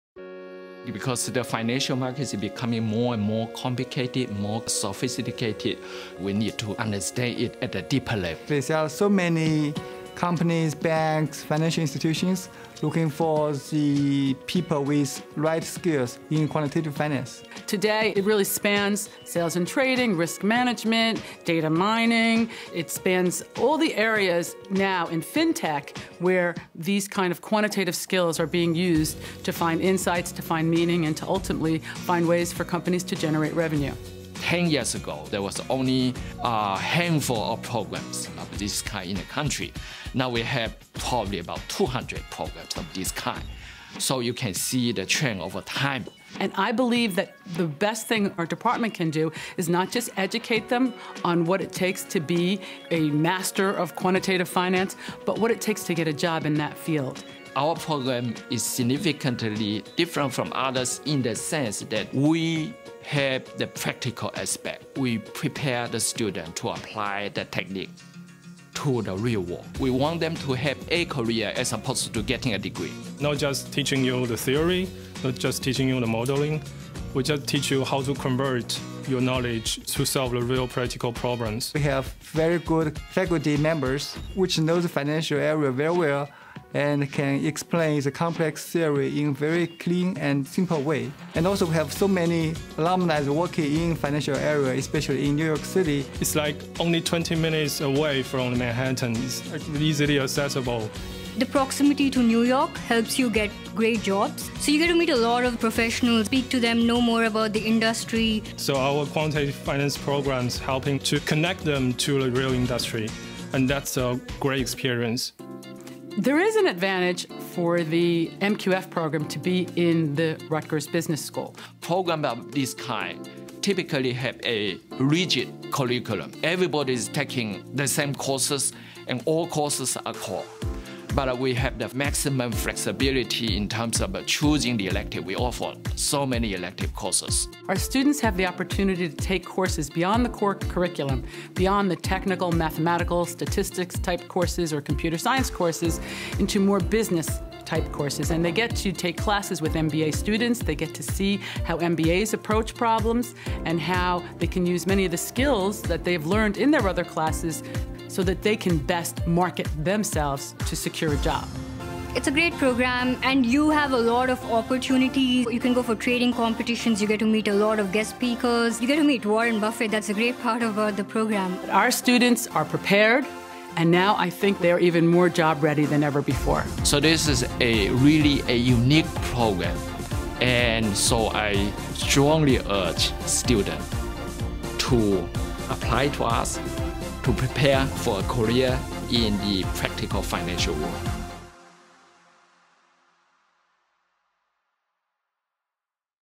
Become a career "Quant" through Rutgers Quantitative Finance program
| Rutgers Master of Quantitative Finance program was ranked a "Top 10 Quant School" by Wall Street executives. Students come from diverse backgrounds and are brought together by a common passion for finance.